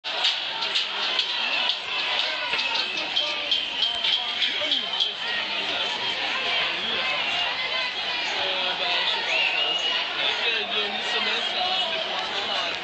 Obreros con el Movimiento Social!!! 28/08/2012
Obreros apoyando al movimiento social desde sus trabajos!!!